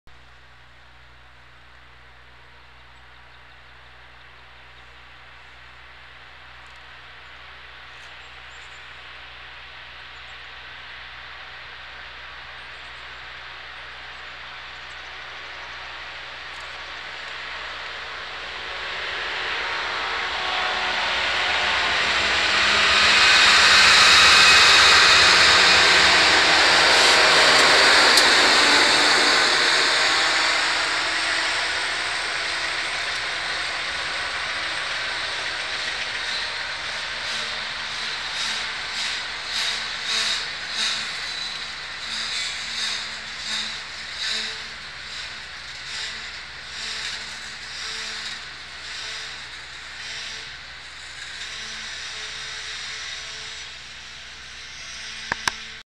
NS 25Z SUMMERHILL, PA.
NS 25Z intermodal train with a NS Dash9 and a NS SD60M pass through Summerhill, PA. on 3/7/12.